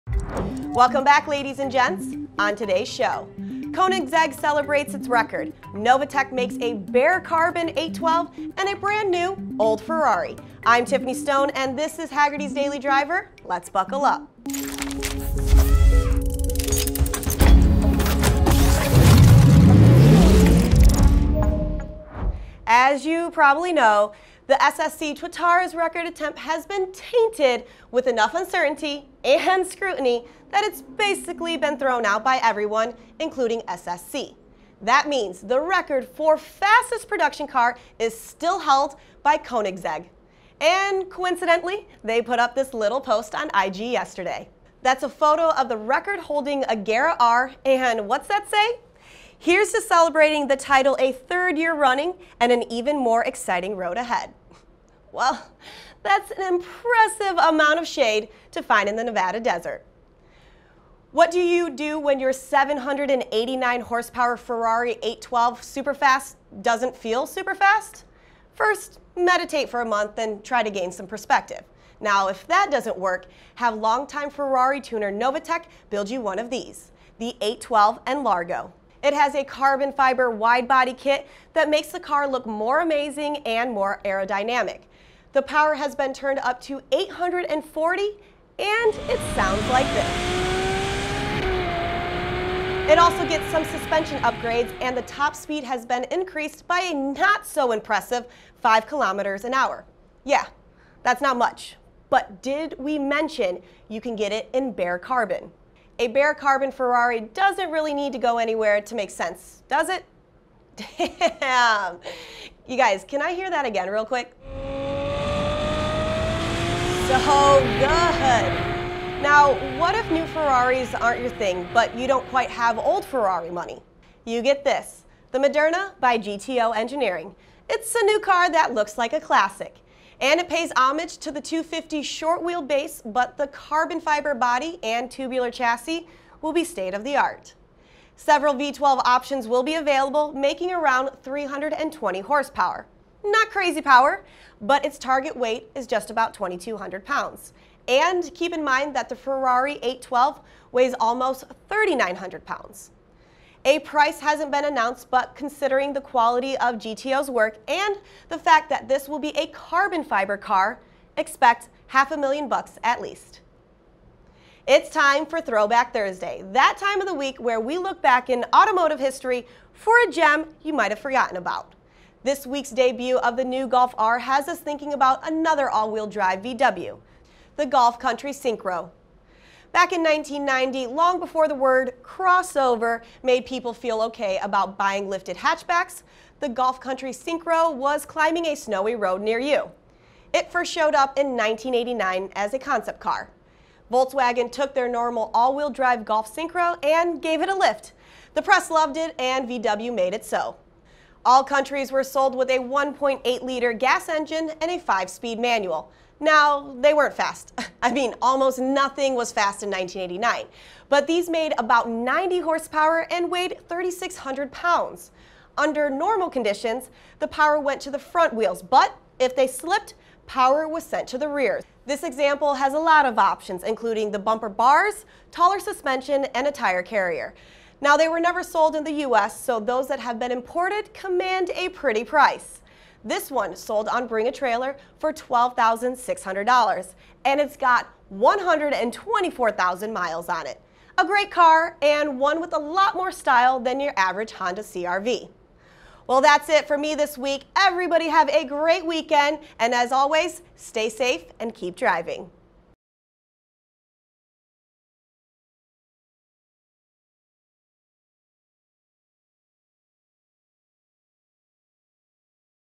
A new-old Ferrari GTO, Novitec 812 N-Largo, Koenigsegg speed record - Daily Driver
Koenigsegg reminds us that the Agera R is still the fastest production car, Novitec reveals a very special bare carbon Ferrari 812, and GTO Engineering's stunningly light Moderna mixes old Ferrari style with new tech. Plus, a look back at the Volkswgen Golf Syncro Country.